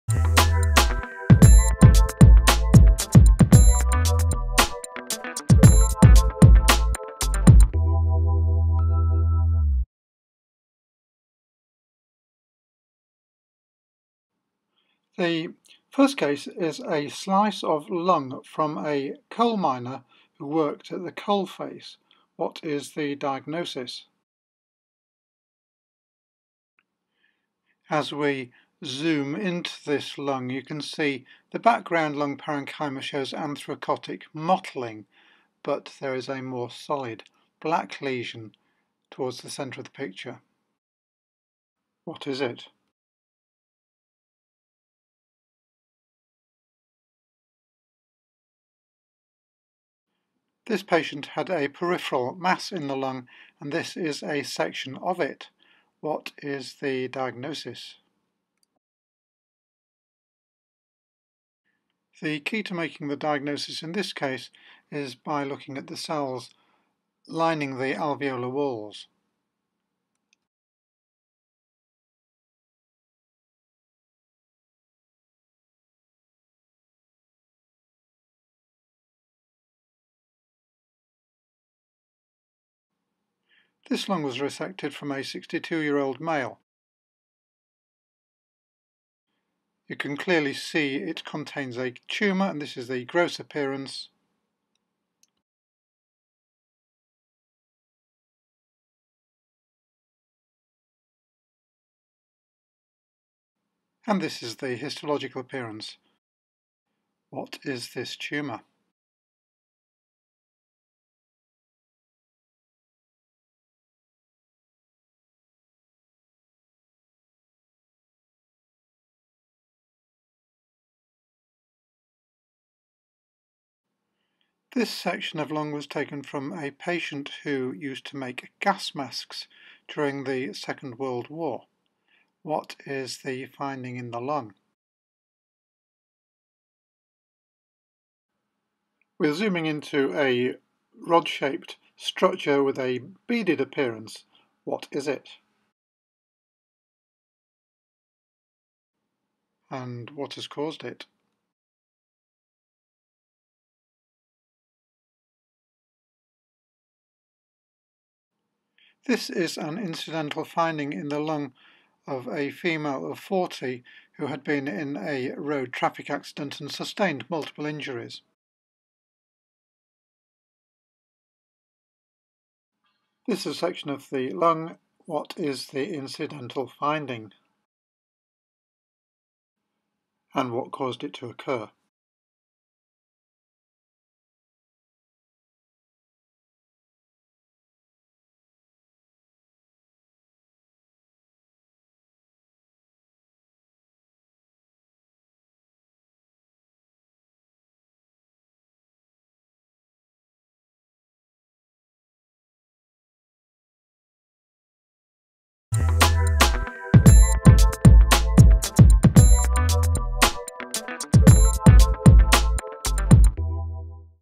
Quiz 15 - Respiratory Pathology
A short quiz with a respiratory theme this time. 5 questions. The answers will be in the next video.